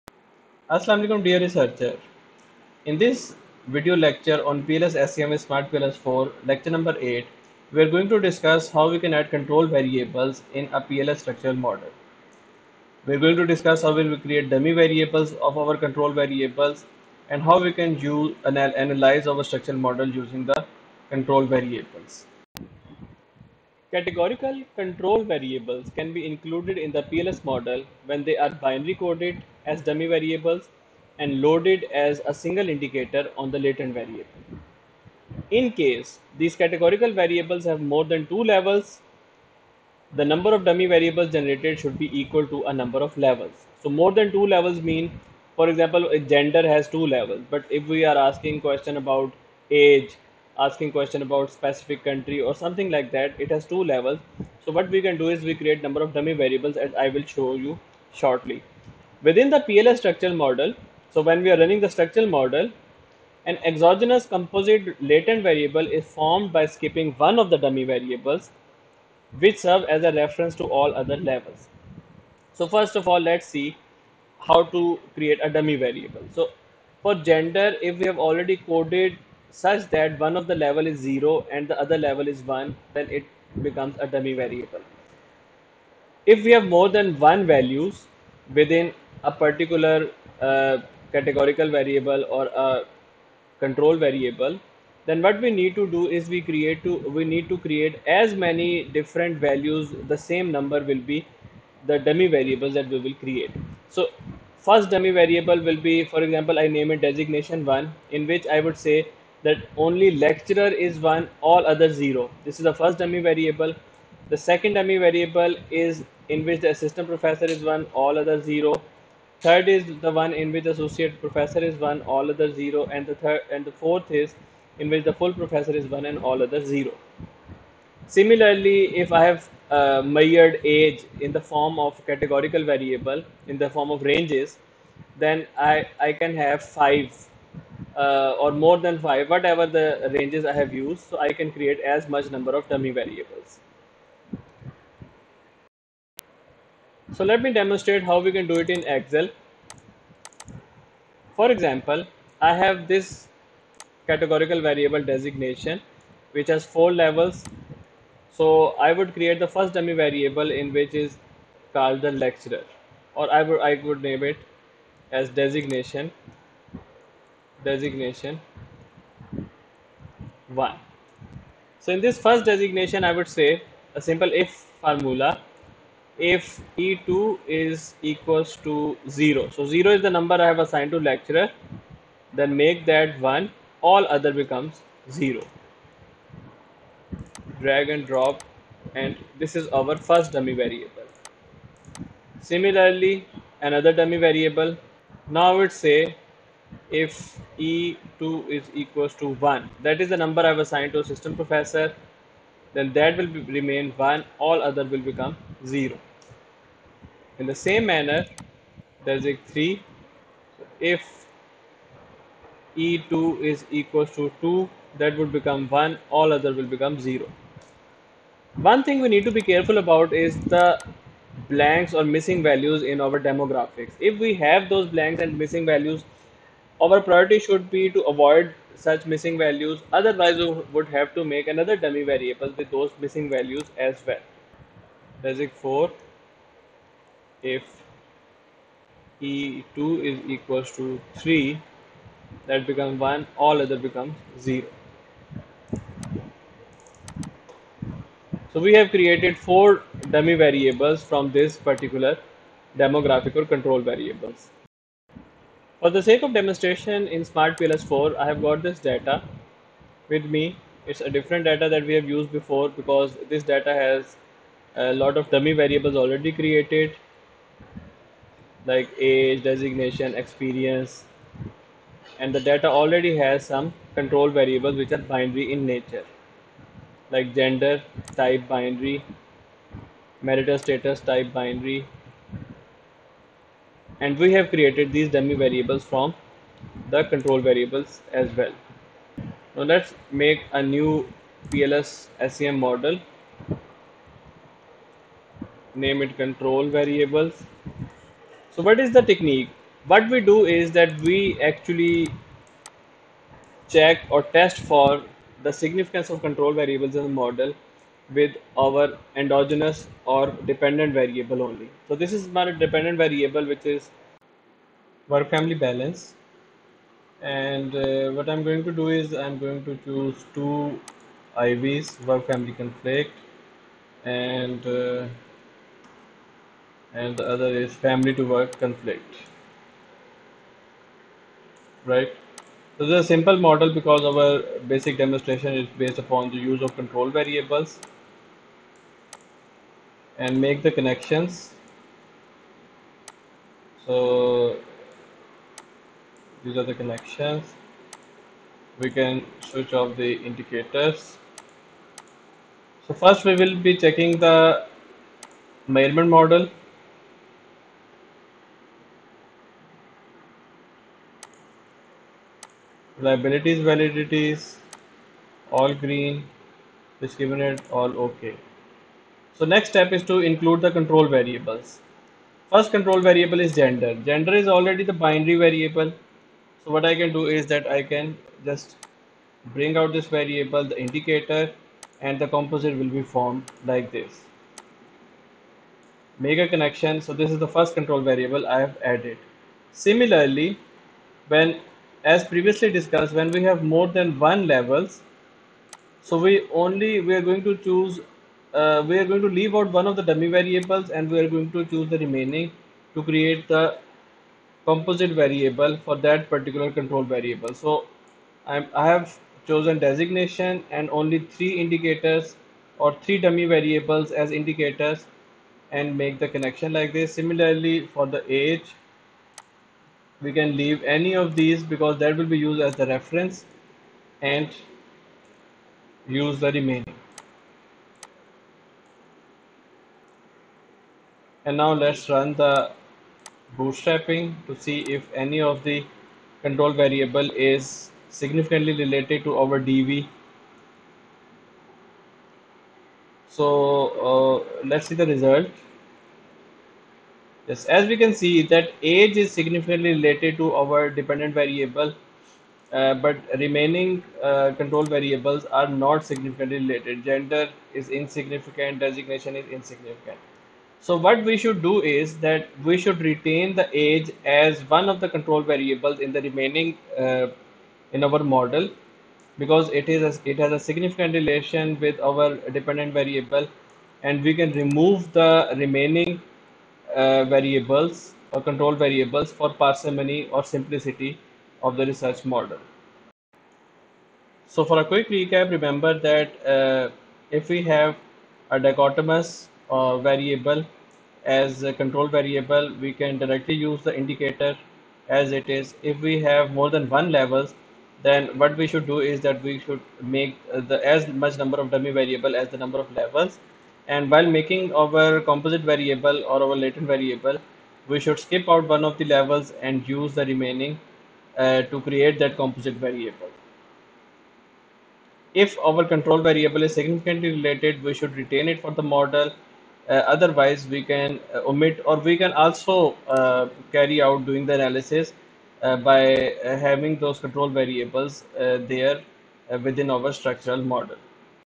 SMARTPLS 4 Lec 8: Analysis of control variable , creating dummy variables
### Summary
In this video lecture focused on PLS SEM (Partial Least Squares Structural Equation Modeling) using SmartPLS 4, the researcher discusses the incorporation of control variables into structural models. Control variables, which are crucial for addressing potential confounding factors, can be included in the model as dummy variables. The lecture provides a step-by-step guide on how to create these dummy variables for categorical data, particularly when dealing with variables that have more than two levels. The researcher illustrates the process using examples from demographics such as gender and designation. After creating the dummy variables, the lecture emphasizes the importance of testing these variables for their significance in relation to the dependent variable, using bootstrapping techniques. The session concludes by highlighting best practices for including control variables in structural models, stressing the need for parsimony and the significance of retained variables in the analysis.

### Highlights
- 📊 **Control Variables Explained:** Understanding the role and importance of control variables in PLS SEM models.
- 🔢 **Dummy Variable Creation:** Step-by-step demonstration on creating dummy variables for categorical control variables.
- 🧑‍🏫 **Categorical Variable Examples:** Practical examples using demographic variables like gender and designation to illustrate the process.
- 📈 **Bootstrapping Techniques:** Utilizing bootstrapping to test the significance of control variables in relation to the dependent variable.
- 📝 **Model Simplification:** Emphasis on retaining only significant control variables for a simpler and clearer model.
- 🔍 **Missing Values Consideration:** Addressing the potential impact of missing values when creating dummy variables.
- 🎓 **Best Practices Recap:** A summary of the best practices for including control variables in structural equation models.

### Key Insights
- 📊 **Importance of Control Variables:** Control variables play a vital role in structural equation modeling as they help to isolate the effects of the main independent variables on the dependent variable. By controlling for these extraneous factors, researchers can obtain more reliable and valid results. Control variables can reduce bias in the estimation of relationships between variables, leading to better insights and conclusions.

- 🔢 **Dummy Variables for Categorical Data:** The lecture illustrates how to convert categorical variables into dummy variables, which allows for their inclusion in regression-based models. This conversion is crucial when dealing with non-binary categorical data, as it enables the representation of multiple levels without losing information. The creation of dummy variables ensures that each category is appropriately accounted for in the analysis.

- 🧑‍🏫 **Examples Enhance Understanding:** Using practical examples, such as gender and academic designations, helps clarify the process of creating dummy variables. By relating theoretical concepts to real-world scenarios, the audience can better comprehend the application of these techniques in their own research.

- 📈 **Significance Testing with Bootstrapping:** The use of bootstrapping to evaluate the significance of control variables adds a robust statistical layer to the analysis. This method allows for assessing the reliability of the findings, ensuring that only those control variables that meaningfully contribute to the model are retained. The results can inform decisions regarding which variables to keep or omit.

- 📝 **Model Parsimony:** The concept of parsimony is emphasized, encouraging researchers to retain only those control variables that demonstrate significant relationships with the dependent variable. This approach not only simplifies the model but also enhances the interpretability of the results. By avoiding unnecessary complexity, researchers can focus on the most impactful variables.

- 🔍 **Addressing Missing Values:** The lecture highlights the importance of managing missing values in demographic data when creating dummy variables. Missing values can skew results and lead to misinterpretation, making it crucial to have a strategy for handling such cases, whether through imputation or exclusion. This attention to detail strengthens the integrity of the analysis.

- 🎓 **Best Practices for Control Variables:** The lecture concludes with a recap of best practices, reinforcing the importance of including control variables in structural models. These practices not only guide researchers in their analysis but also contribute to the overall quality and credibility of their findings. Emphasizing the significance of retaining meaningful control variables provides a pathway for more focused and impactful research outcomes.